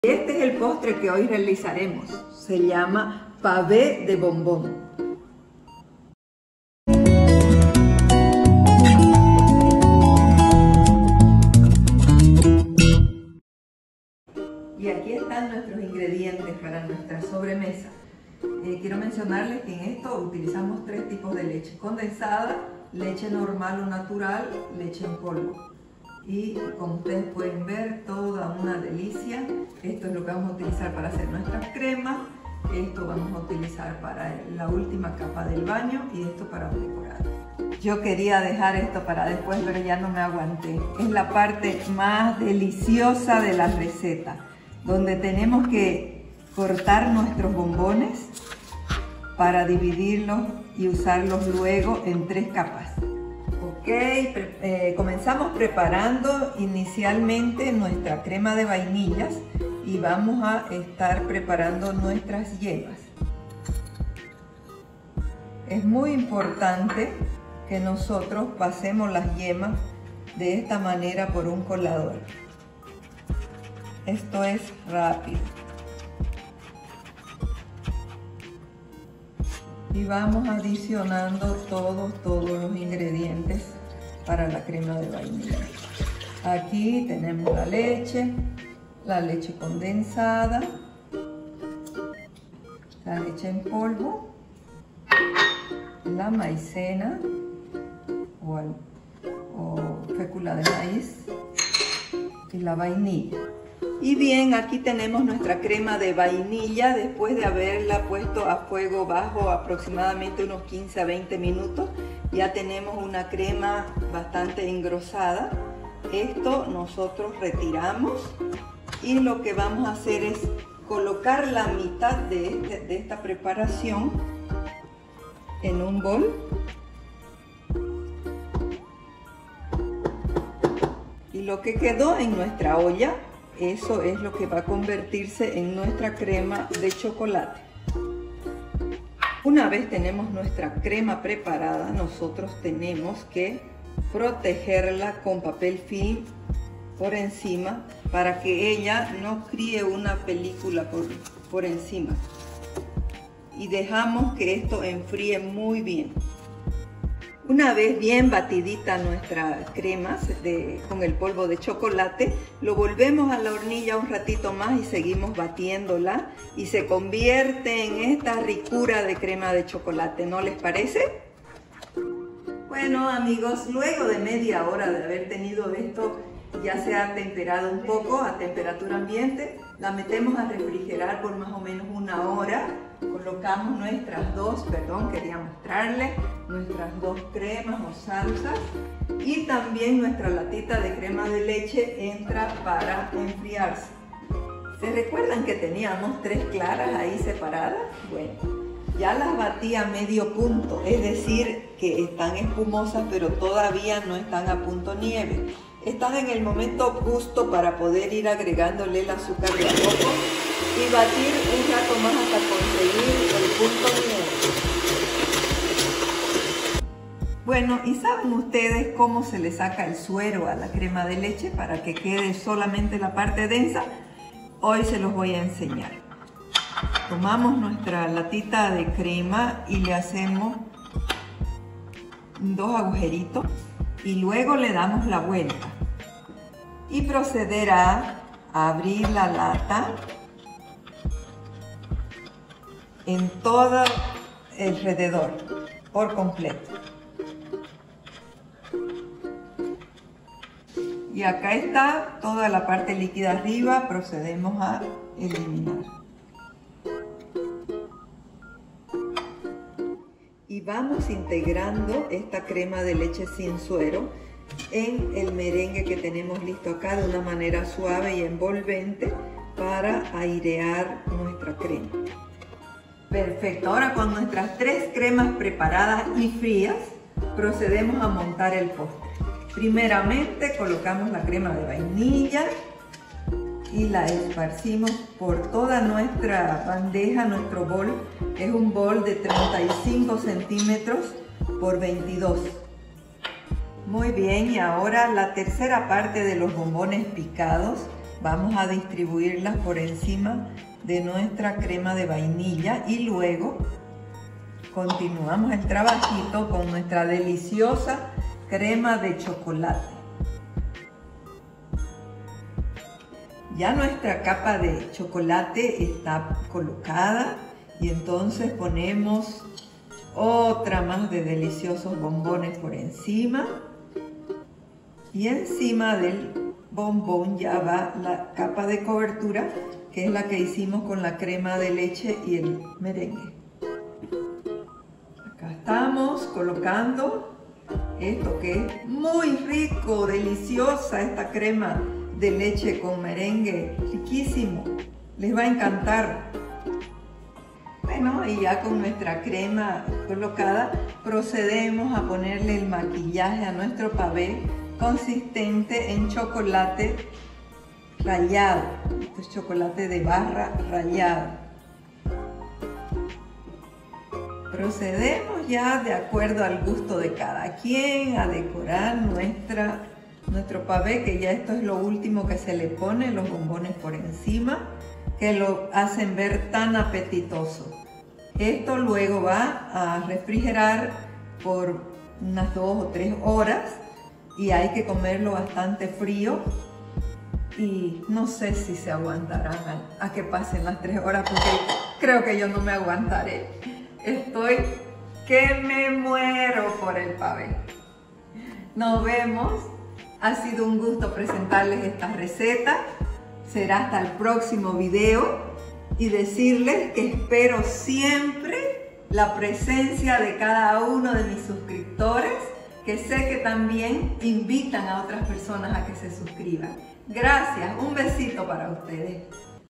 Pavé de Bombón: delicioso postre brasilero 😍
¡Te aseguro que este se volverá tu postre favorito!!!

INGREDIENTES:

15 unidades de bombón
3 yemas de huevo
2 latas de leche condensada
1/2 taza de leche en polvo
1 cda. de vainilla líquida
3 tazas de leche (600 ml.)
5 cdas. de fécula de maíz (maicena)
3/4 taza de chocolate en polvo
3 claras de huevo
6 cdas. de azúcar
1 lata de crema de leche sin suero
1 tableta de chocolate semi amargo

Si tienes dudas o sugerencias, déjalas en los comentarios para poder responderte.
Pronto subiré más videos, tengo muchas recetas y tips para compartirte...